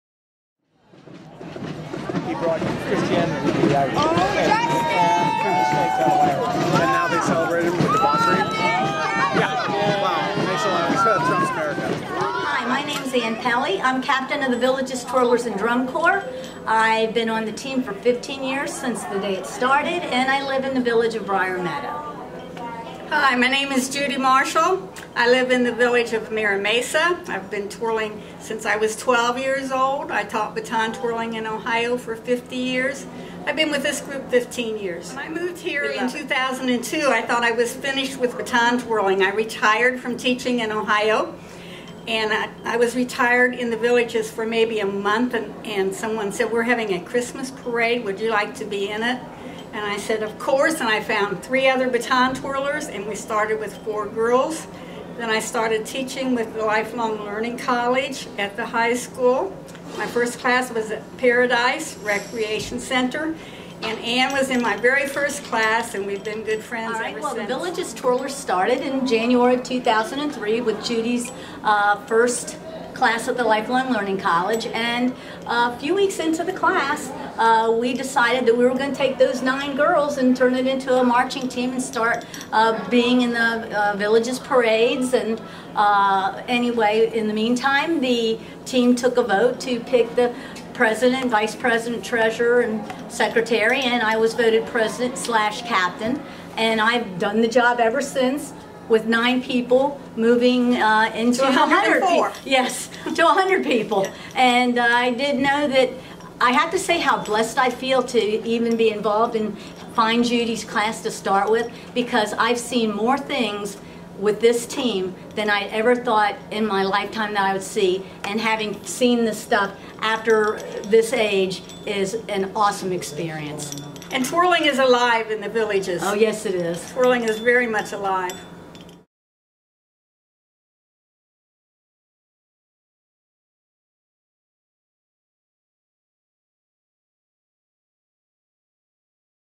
Villages Twirlers celebrate 15-year anniversary
The Villages Twirlers celebrated their 15-year anniversary at La Hacienda Recreation Center in The Villages.